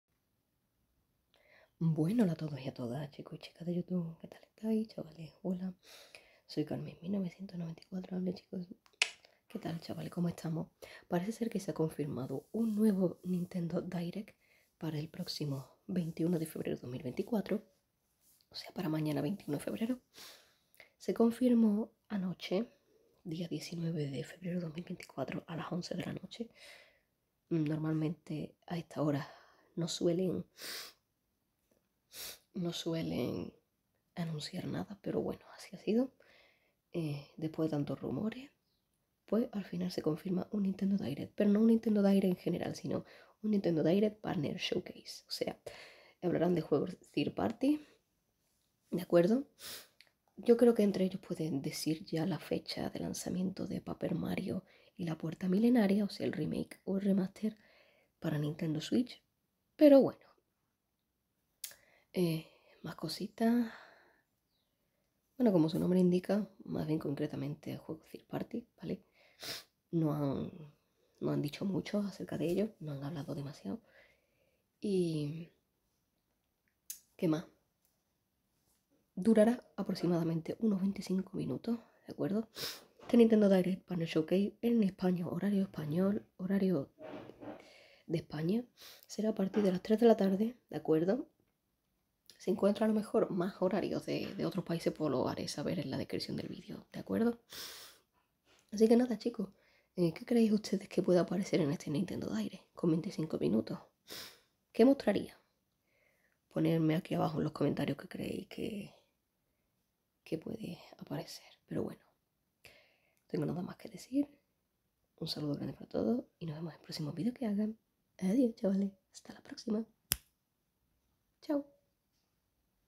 ¡ANUNCIADO NUEVO NINTENDO DIRECT PARA MAÑANA 21 DE FEBRERO DE 2024!
Anunciado nuevo Nintendo Direct para mañana, 21 de febrero a las 15:00 hora española, durará 25 minutos.

Disfrutenlo!!!